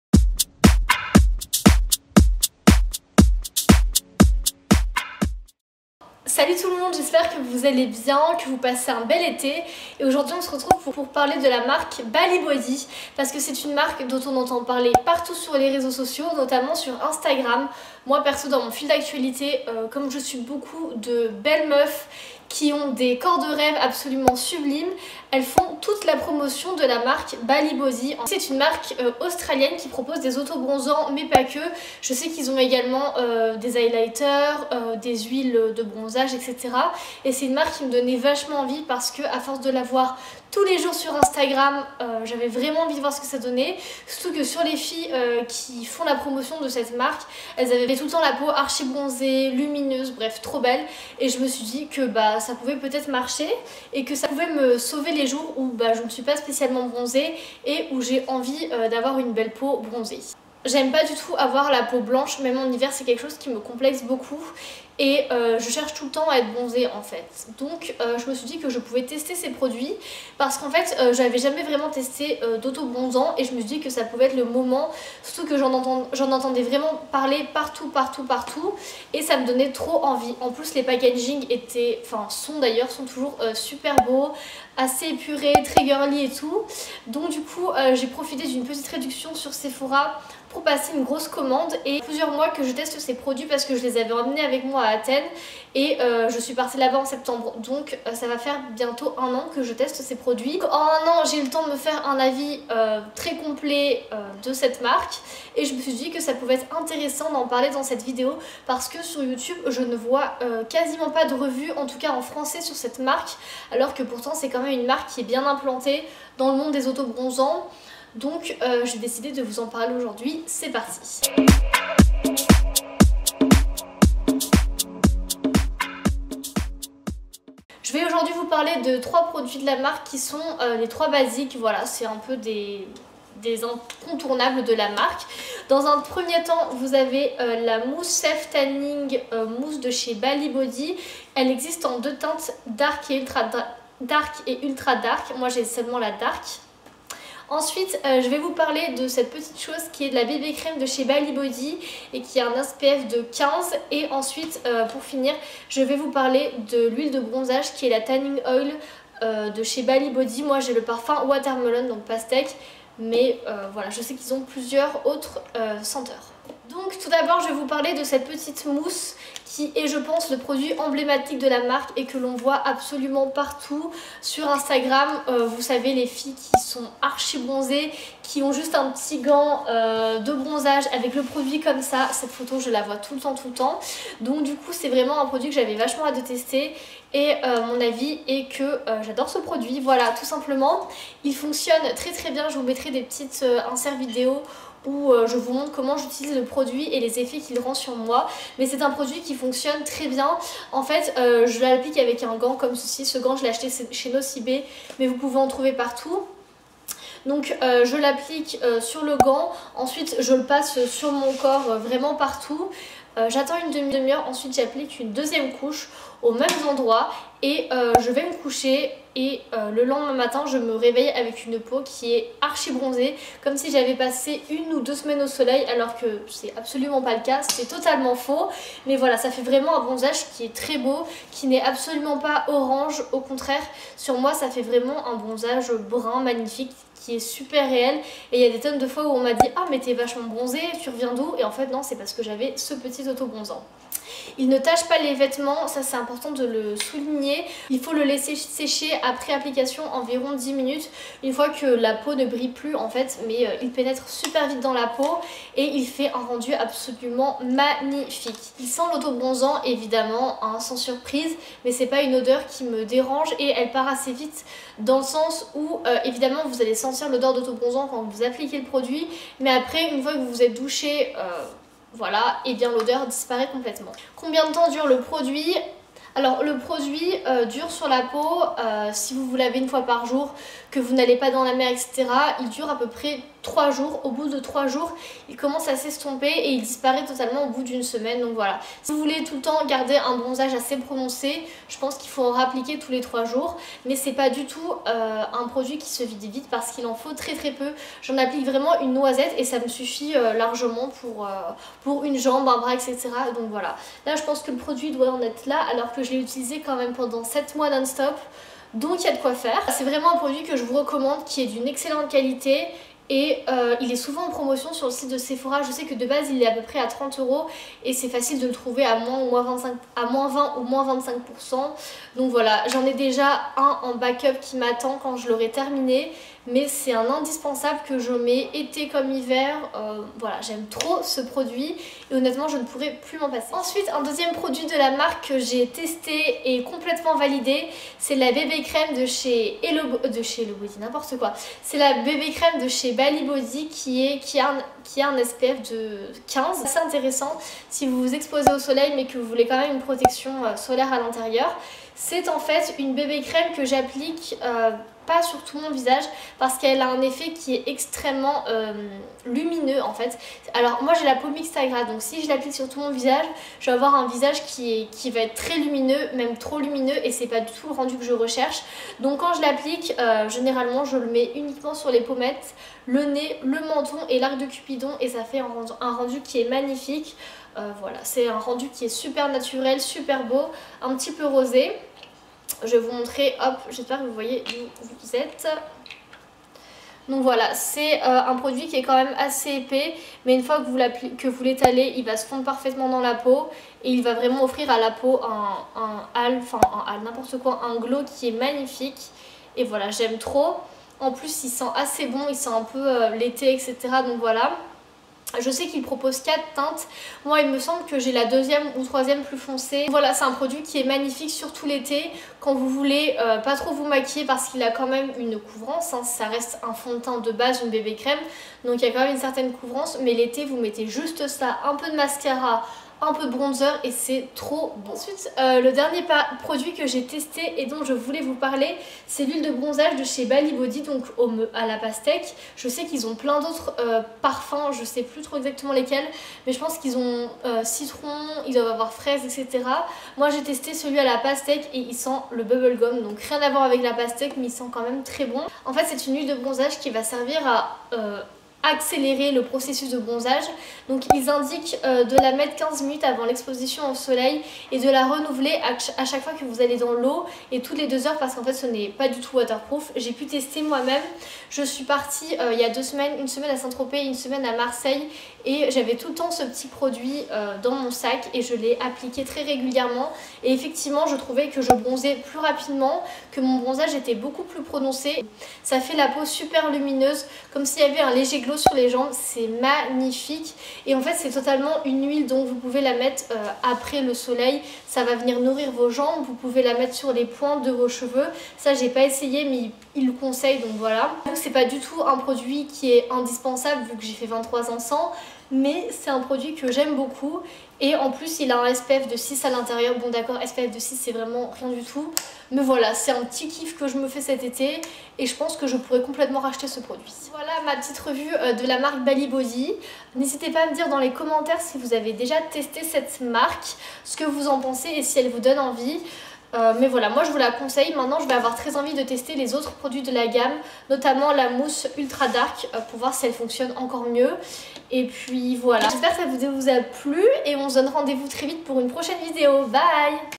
BALI BODY REVUE⎟QUE VAUT LA MARQUE?
Hello! Aujourd'hui on se retrouve pour une revue avec mon avis honnête sur les produits de la fameuse marque d'auto bronzants Bali Body.

Love, Mathilde